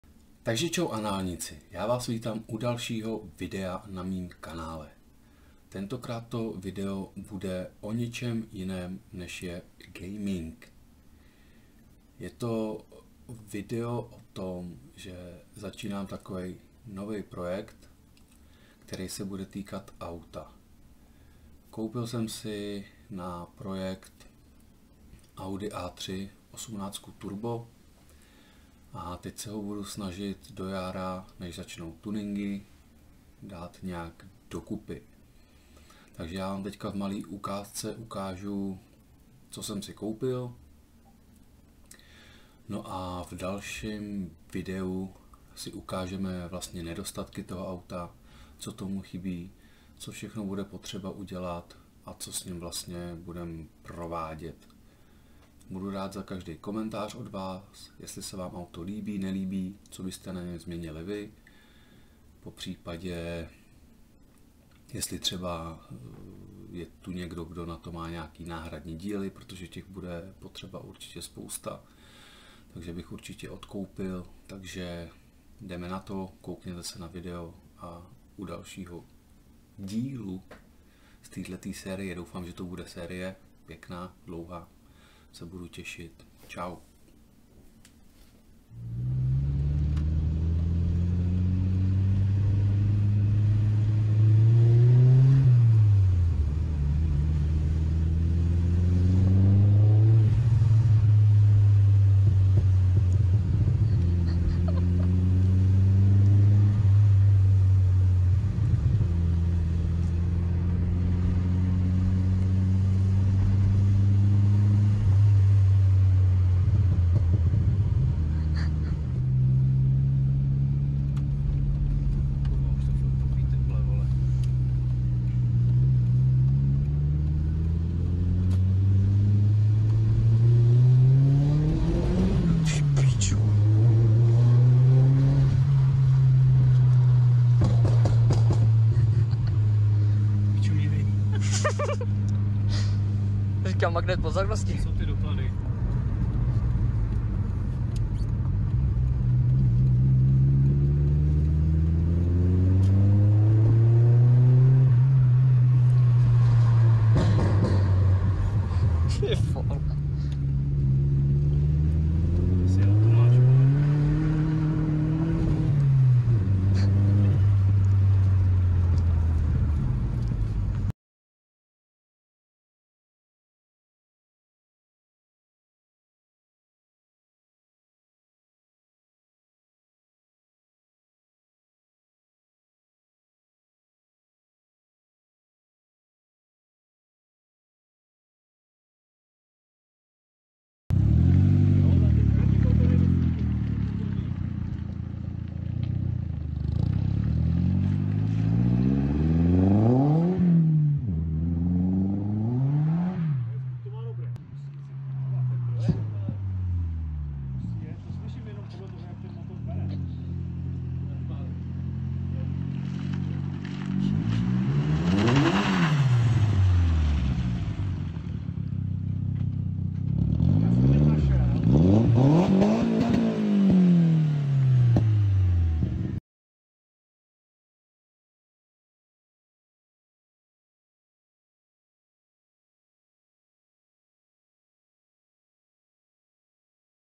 AUDI A3 1,8t - My project Car
Takže čau, po dlouhé době jsem se sem odvážil dát video s mým novým projektem, je to audi A3 1,8t. Je na něm mnoho práce, i když to z videa není moc poznat.Takže v dalších videích si auto důkladně projdeme, zjistíme co je potřeba vše opravit, udělat, potunit aby auto do jara bylo jak ze škatulky.